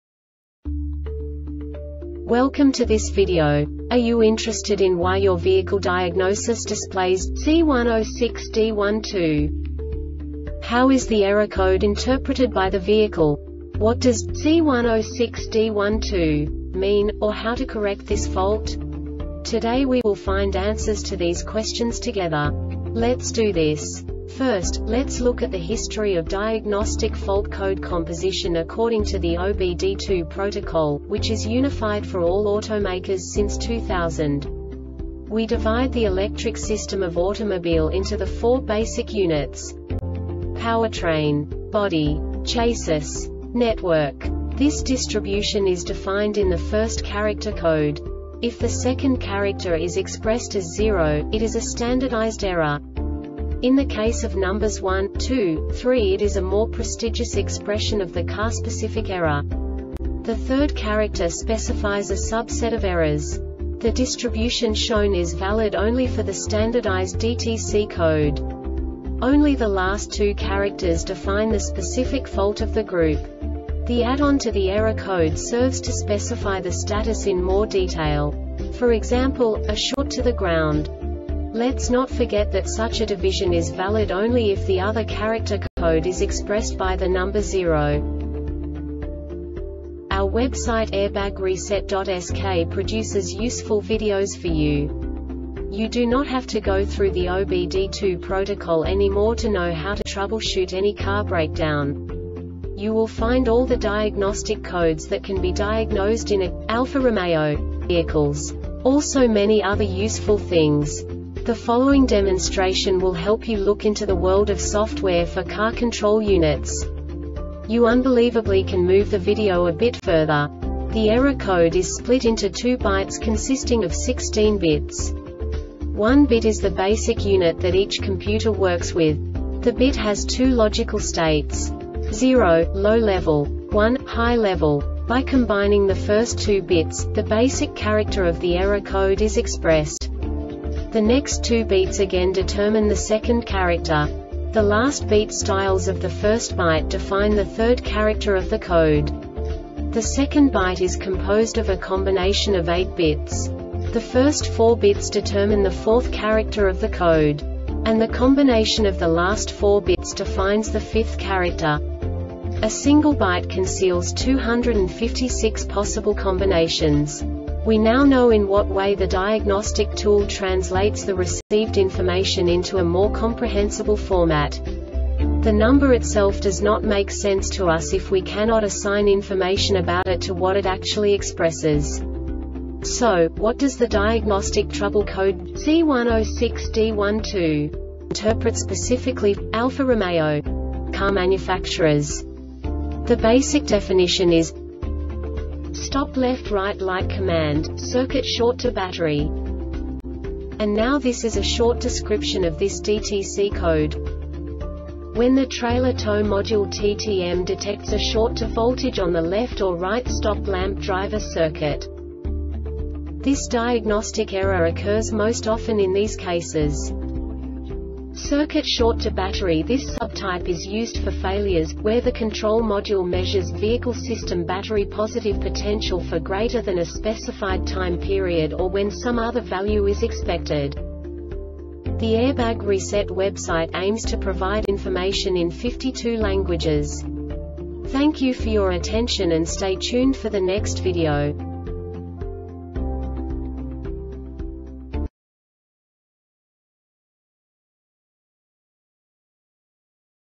DTC Alfa-Romeo C106D-12 Short Explanation
Contents:
0:21 Basic DTC analysis according to OBD2 protocol standard.
1:48 Insight into programming
2:58 A diagnostically understandable description of the Diagnostic Trouble Code
3:05 Basic error definition

Make:
Alfa Romeo
Code:
C106D-12
Definition:
Stop Left Right Light Command - Circuit Short To Battery
Description:
When the Trailer Tow Module (TTM) detects a short to voltage on the Left or Right Stop Lamp Driver circuit.